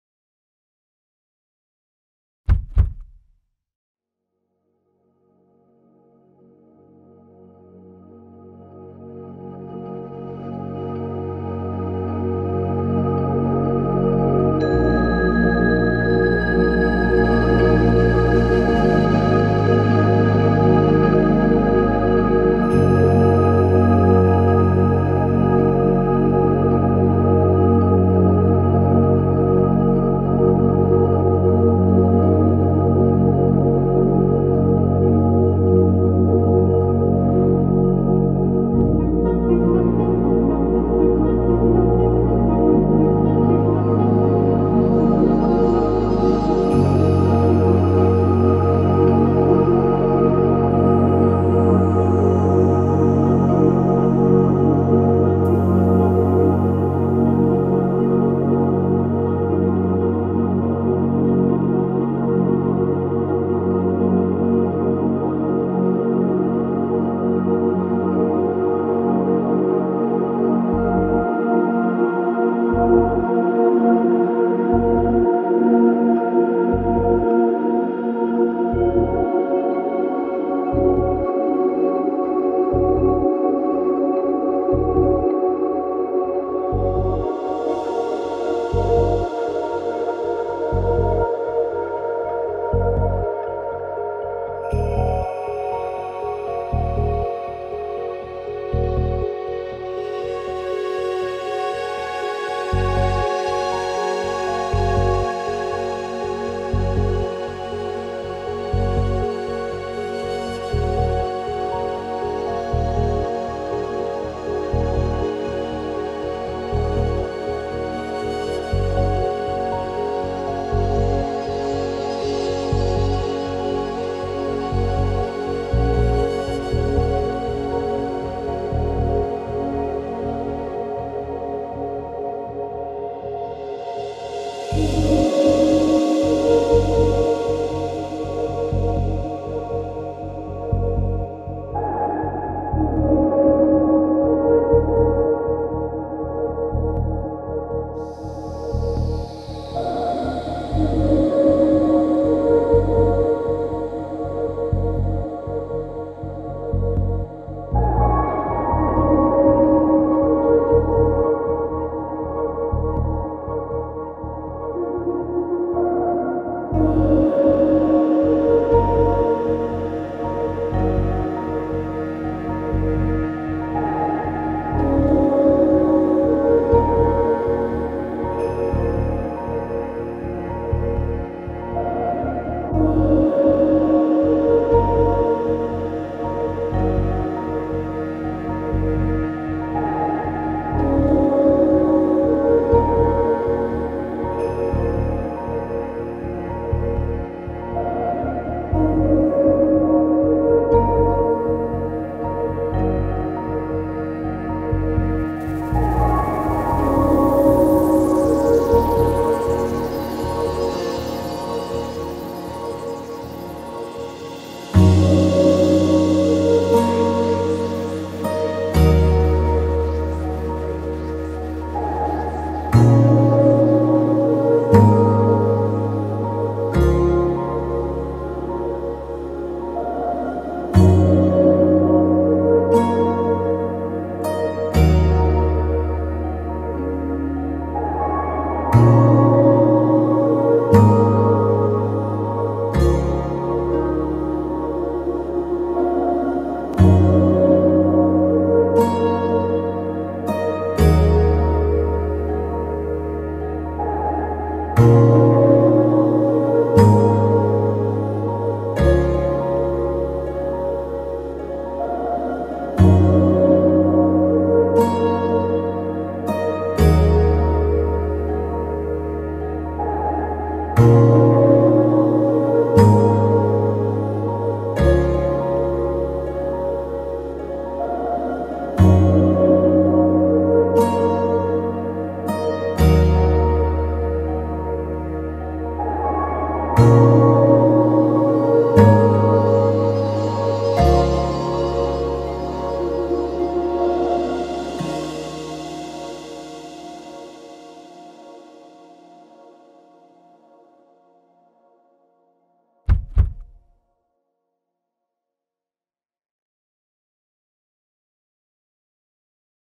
The Making Of A Stamper - A Stamper Discs Film
A short video documenting the stamper manufacturing process at Stamper Discs brand new facility in Sheffield UK.
Shot by cinematographer Max Smith.